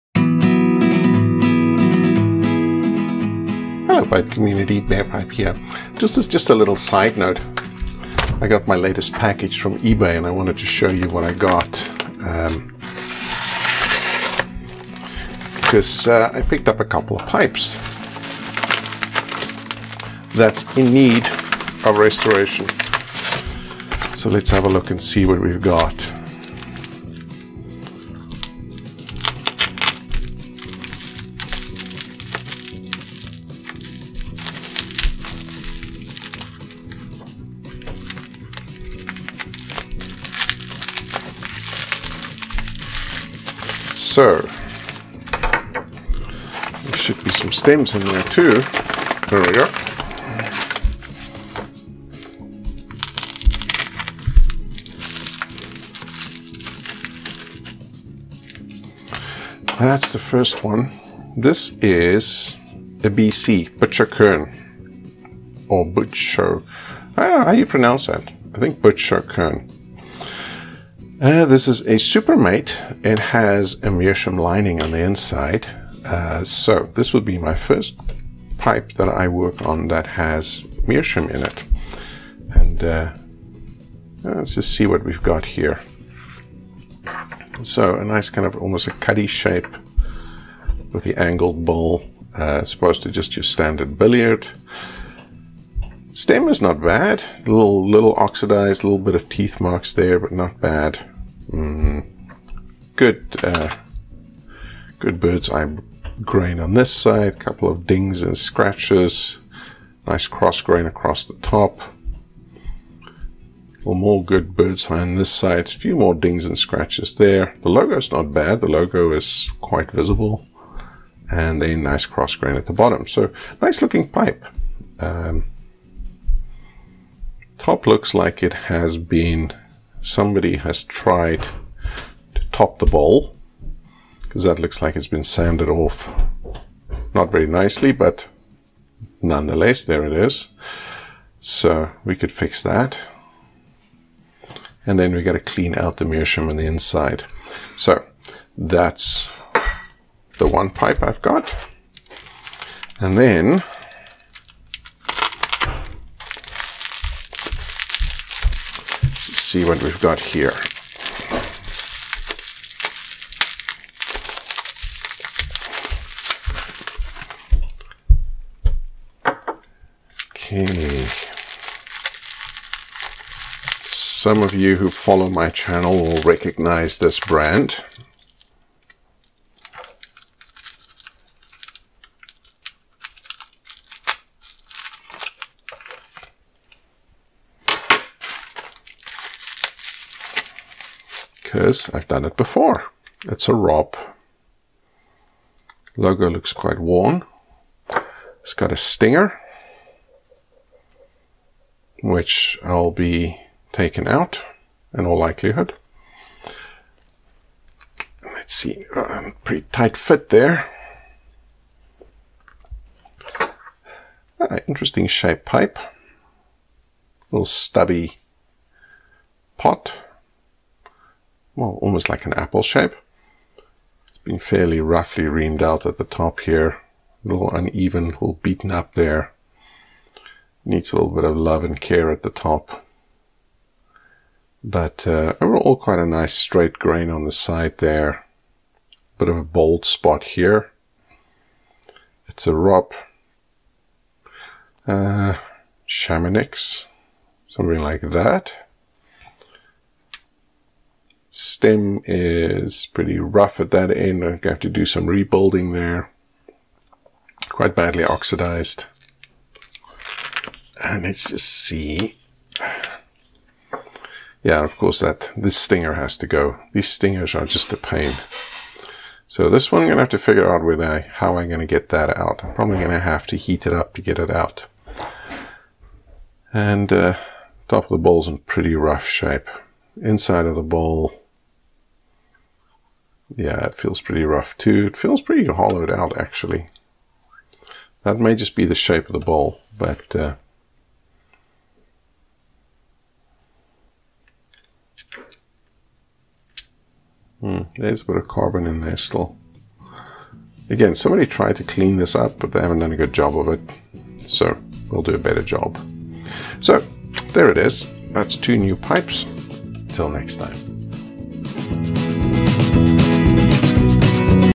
Sidenote 2: Fresh pipes on the bench
June 30 2018: A side note to the regular episodes

Checking out some freshly arrived estate pipes on my bench

The Barepipe experiments with and explores tobacco pipe culture, history and restoration, using basic tools and simple techniques available to an average person